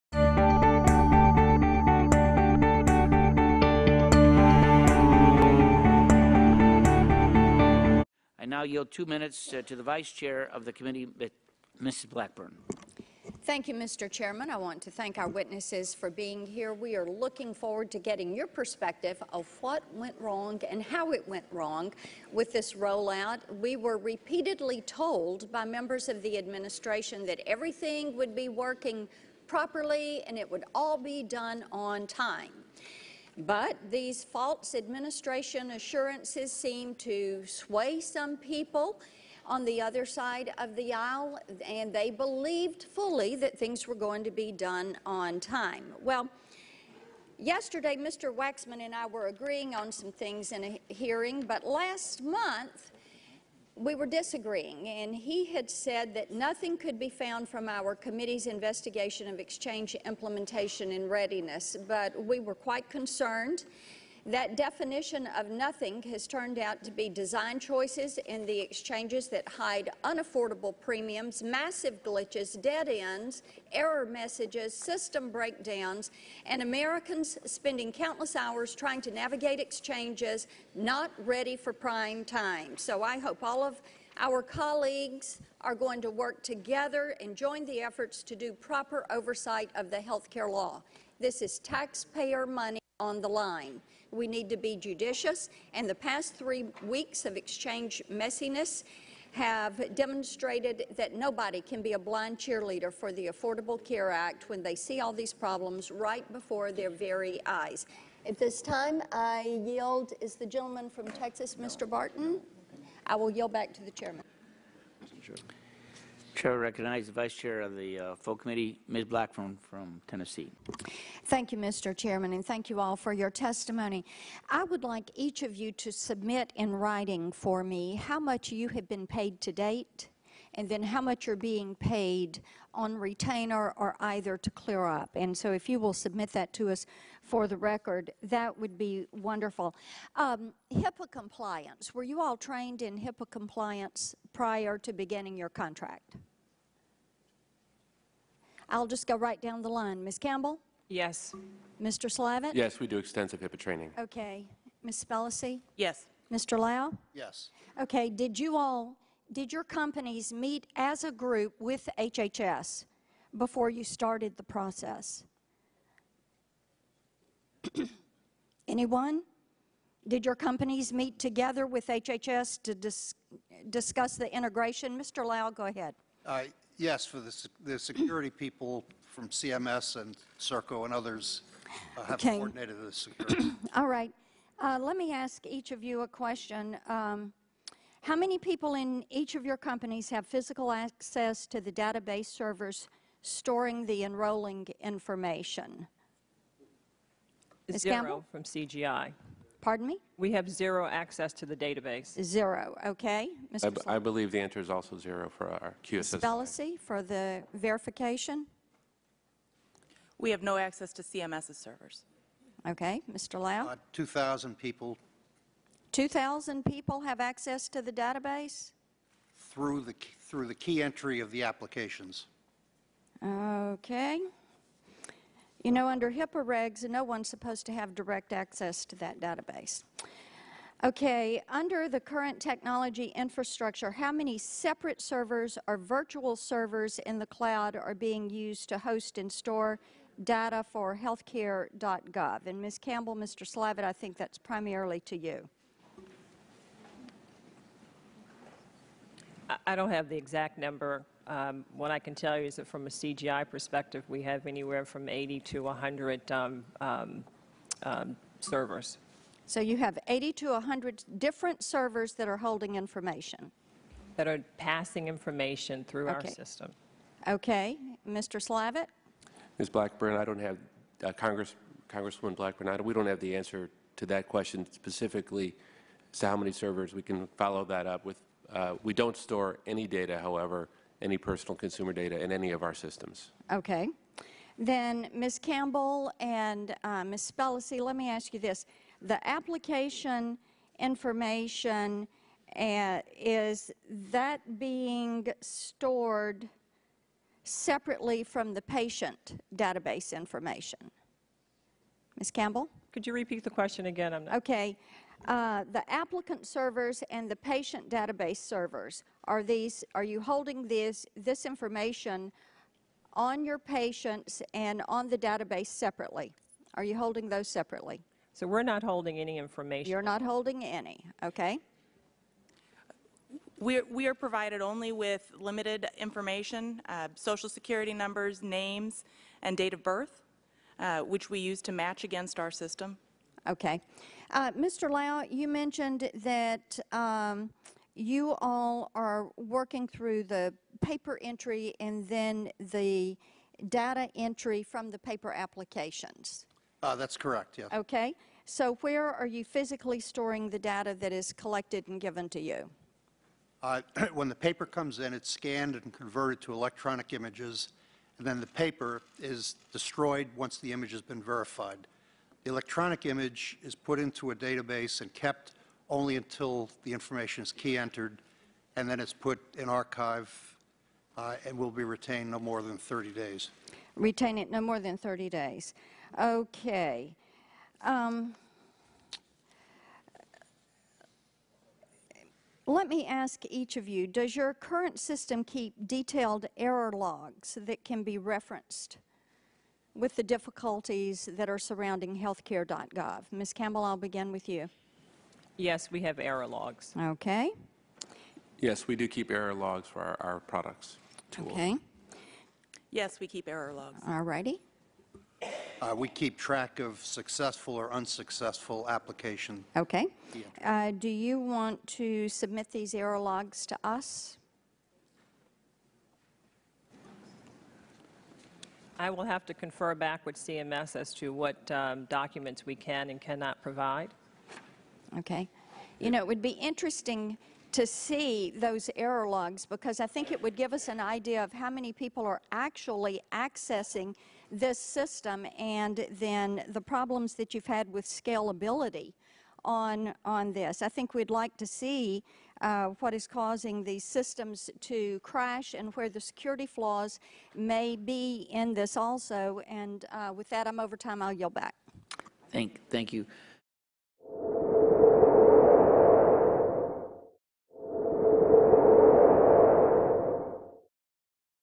Blackburn Seeks Answers to Obamacare Implementation Failures
House Energy and Commerce Committee Vice Chair Marsha Blackburn investigates what went wrong with HealthCare.gov at the committee's hearing examining the failed Obamacare rollout. (October 24, 2013)